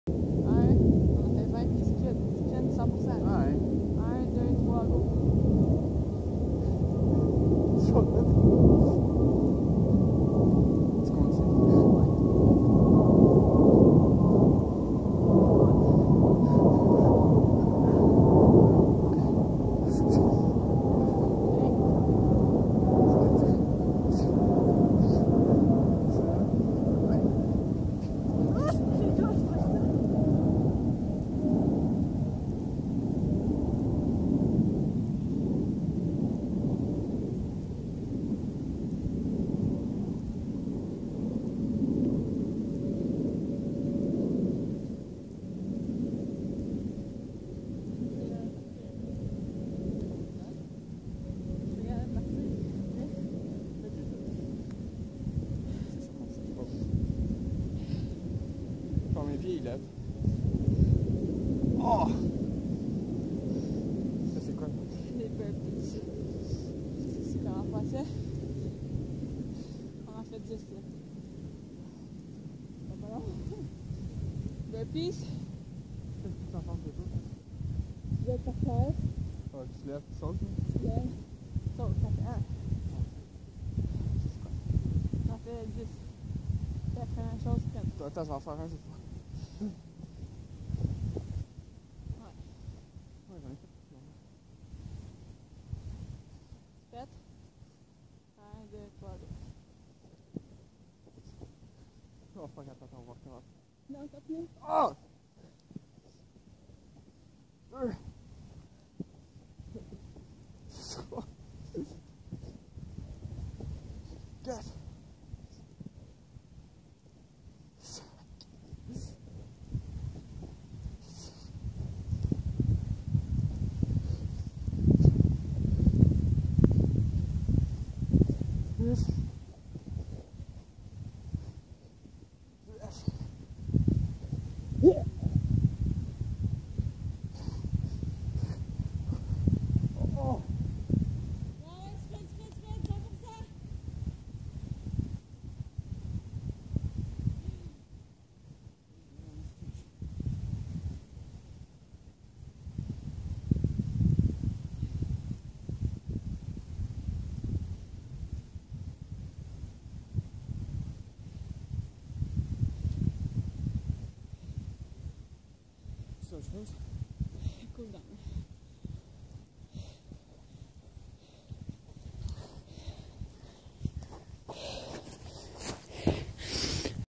Laboratoire de situations motrices | Défi difficile
Pour le cours Intervention psychologique et apprentissage moteur en développement de l'expertise sportive, voici l'exercice qui était trop difficile pour les capacités du participant.